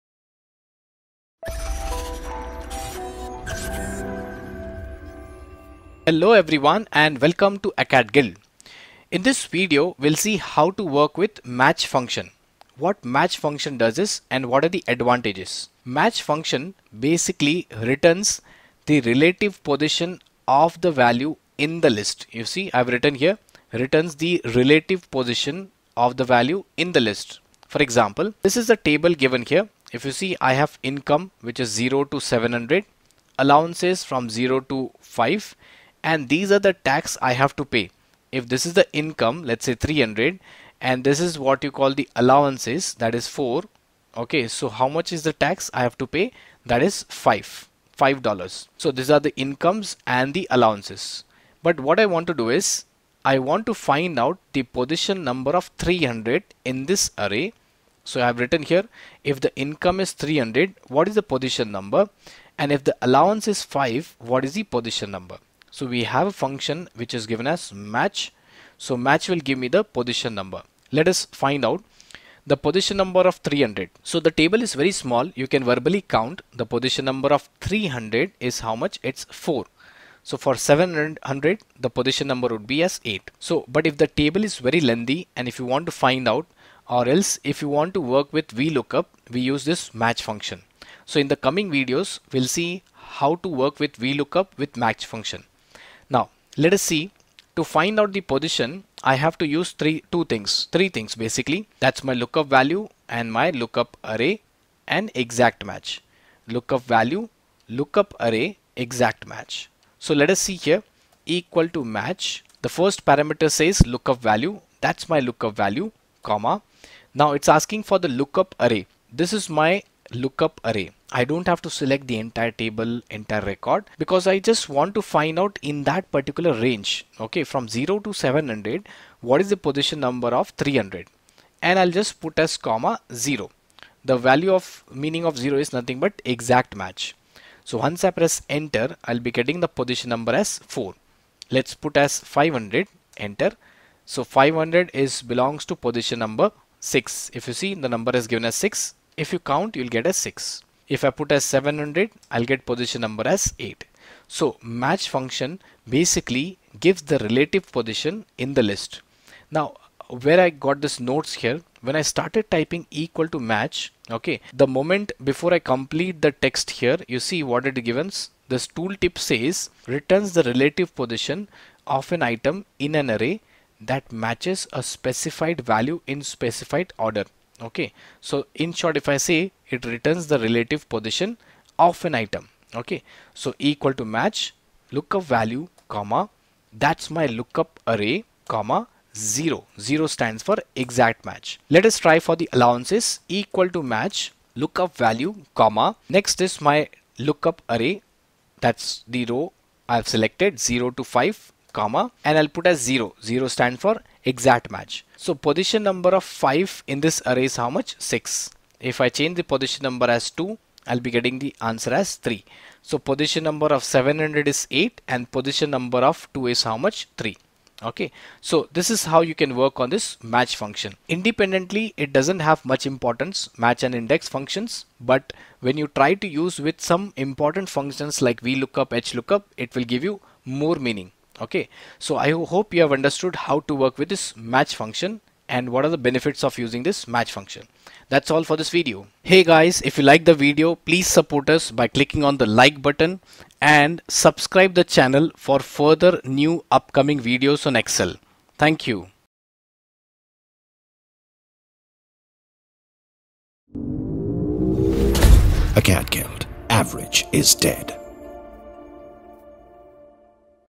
How to Use Match Function in Excel | Excel Tutorial for Beginners
Hello and welcome back to excel tutorials for beginners 2018 powered by Acadgild. In this excel tutorial video, you will be able to learn, how to work with Match function in excel.
In the earlier videos we have seen how to use VLOOKUP and HLOOKUP.  In this video we will see how to work with match functions in excel, specifically, what match function does and its advantages. Match function returns the relative position of the value in the list.